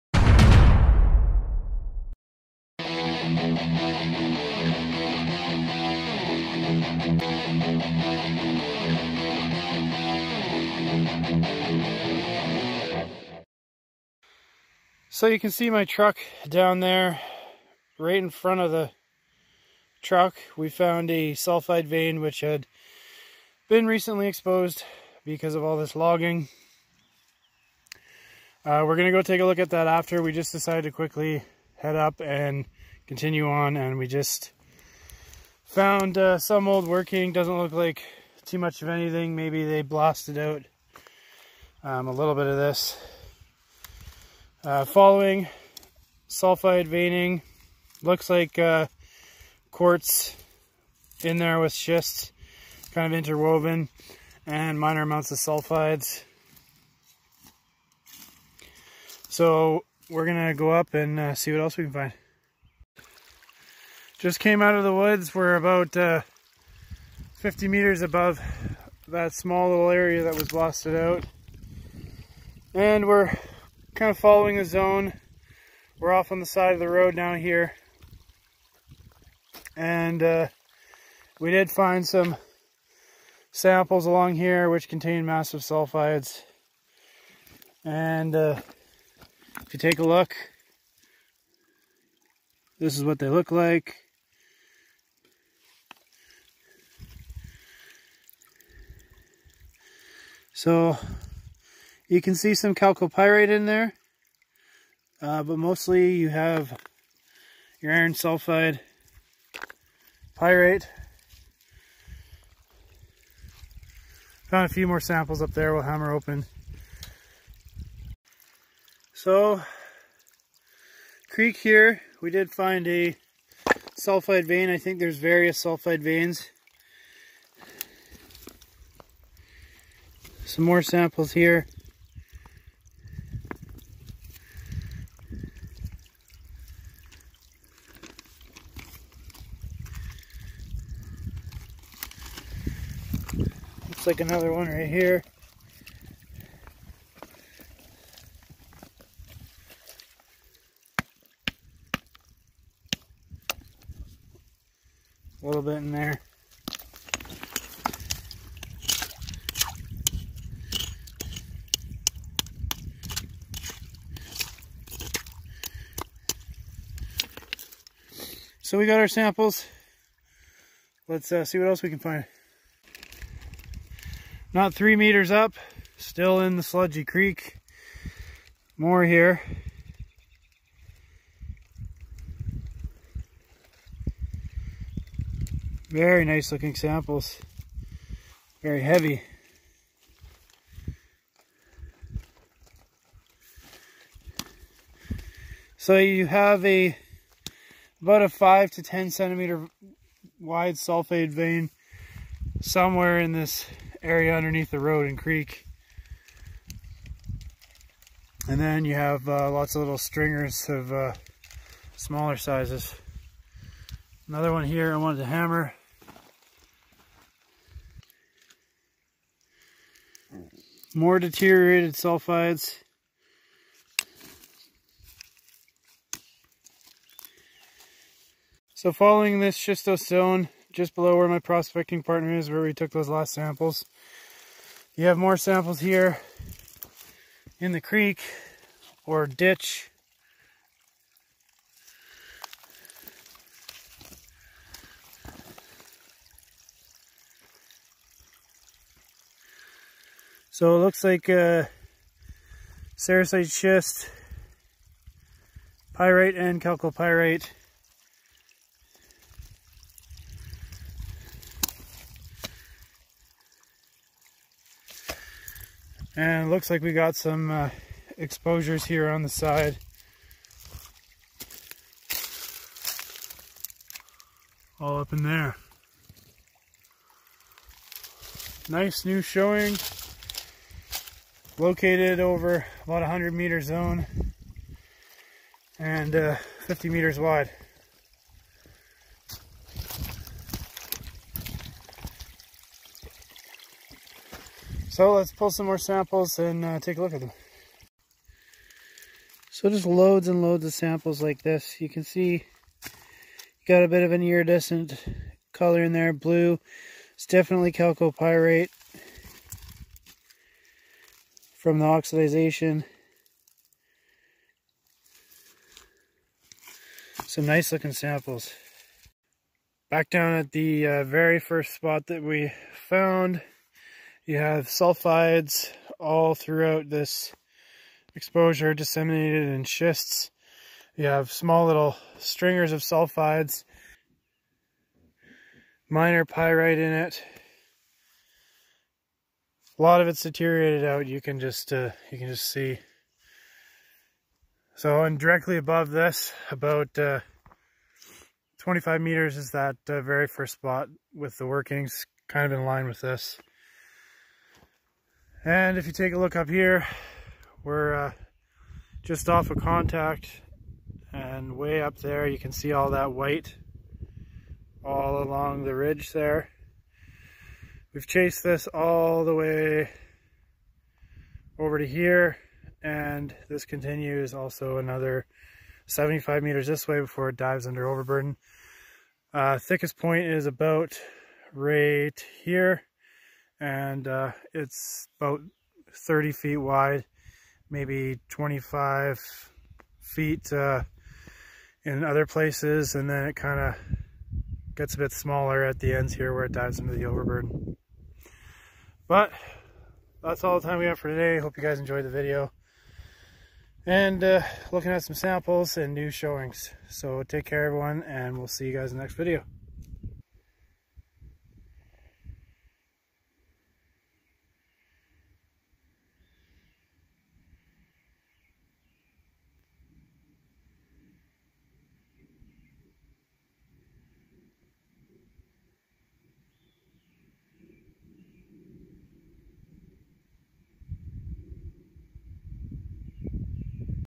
Iron Sulfide Zone In Schist Rock Showing Signs Of Promise!
A zone we found containing sulfide veins and iron sulfides in quartz is showing promise that there could be something bigger. As multiple veins with massive sulfides are found in a 100 meter wide area. These veins are most likely gold bearing and will contain gold, silver, copper and possibly lead and zinc.

VIDEOS FROM OUR MAIN CHANNEL

911 Mining & Prospecting Co